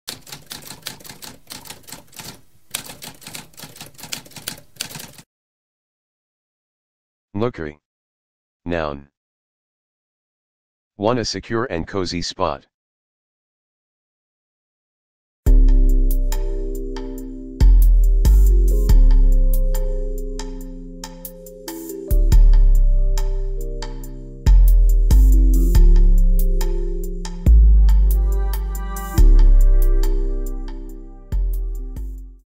What does nookery mean?
A spoken definition of nookery.

Intro Sound:
Typewriter - Tamskp